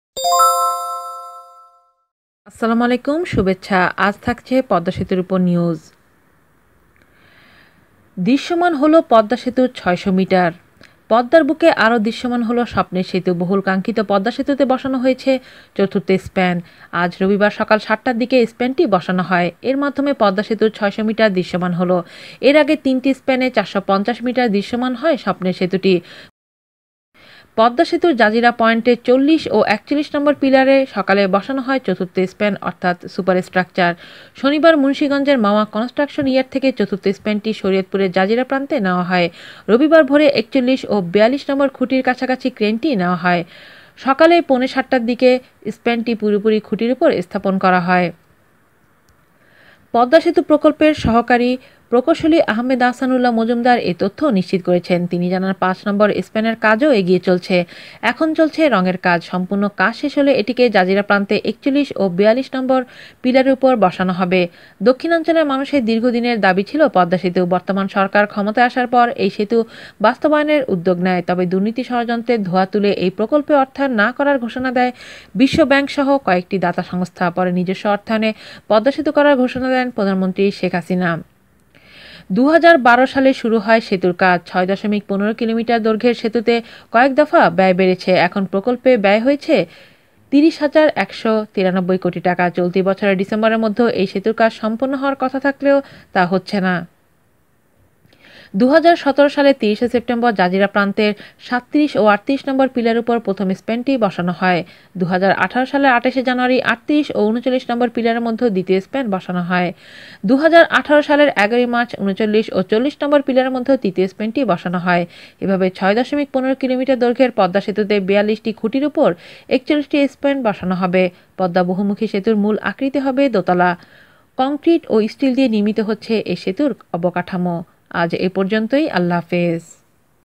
আজ রবিবার সকাল সাতটার দিকে স্প্যানটি বসানো হয়, দৃশ্যমান হলো পদ্মা সেতুর ৬০০ মিটার! Padma Bridge!!
আজ রবিবার সকাল সাতটার দিকে স্প্যানটি বসানো হয়। এর মাধ্যমে পদ্মা সেতুর ৬০০ মিটার দৃশ্যমান হলো। এর আগে তিনটি স্প্যানে ৪৫০ মিটার দৃশ্যমান হয় স্বপ্নের সেতুটি। পদ্মা সেতুর জাজিরা পয়েন্টে ৪০ ও ৪১ নম্বর পিলারে সকালে বসানো হয় চতুর্থ স্প্যান (সুপার স্ট্রাকচার)।

শনিবার মুন্সীগঞ্জের মাওয়া কন্সস্ট্রাকশন ইয়ার্ড থেকে চতুর্থ স্প্যানটি শরিয়তপুরের জাজিরা প্রান্তে নেয়া হয়। রবিবার ভোরে ৪১ ও ৪২ নম্বর খুঁটির কাছাকাছি ক্রেনটি নেয়া হয়। সকাল পৌনে সাতটার দিকে স্প্যানটি পুরোপুরি খুঁটির ওপর স্থাপন করা হয়।

পদ্মা সেতু প্রকল্পের সহকারী প্রকৌশলী আহমেদ আহসান উল্লাহ মজুমদার এ তথ্য নিশ্চিত করেছেন। তিনি জানান, পাঁচ নম্বর স্প্যানের কাজও এগিয়ে চলছে। এখন চলছে রংয়ের কাজ। সম্পূর্ণ কাজ শেষ হলে এটিকে জাজিরা প্রান্তে ৪১ ও ৪২ নম্বর পিলারে বসানো হবে।